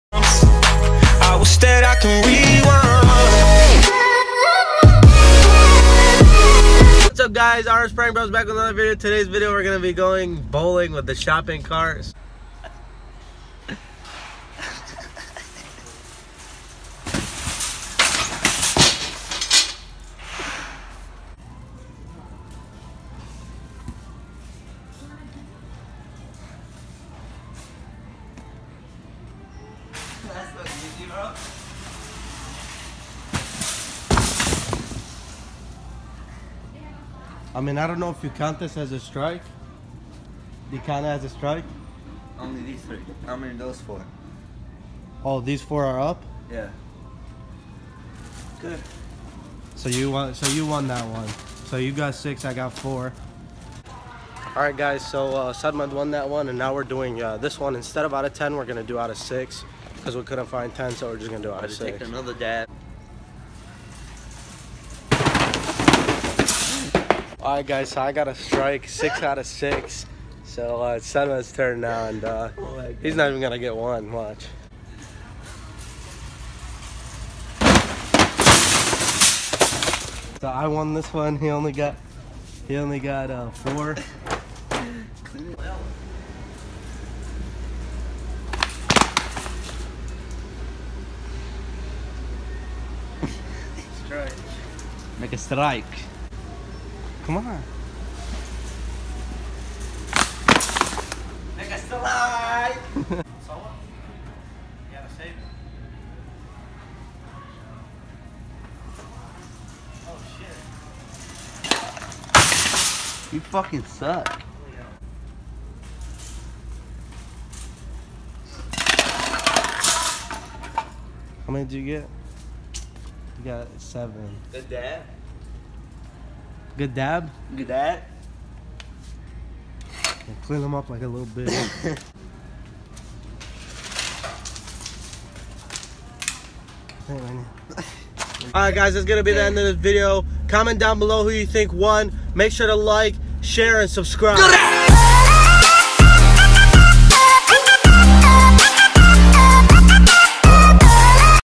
SHOPPING CART BOWLING IN WALMART!
In today’s video we went bowling with a Walmart shopping cart hope you guys enjoy!!

Raphael_0620
Sarmad.bash

Raphaeldoe
Sarmad.bash